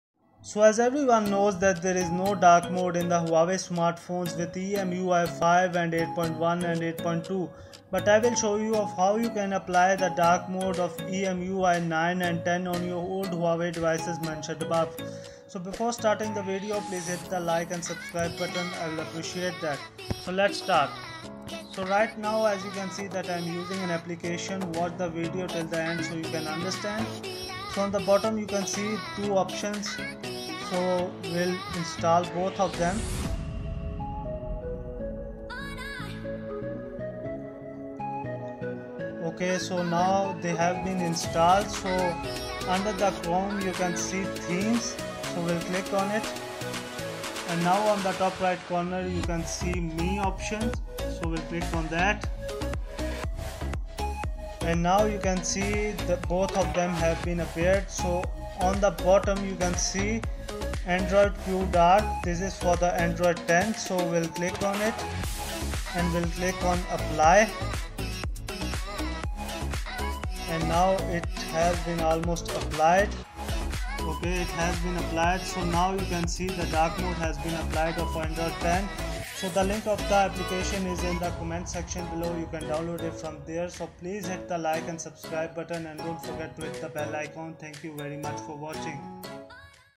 How To Enable Dark Mode Theme In Huawei EMUI 5 and 8 | Software Mobile Tips
I'll Show You How You Can Enable Dark Mode on all Huawei Devices Above "EMUI 5".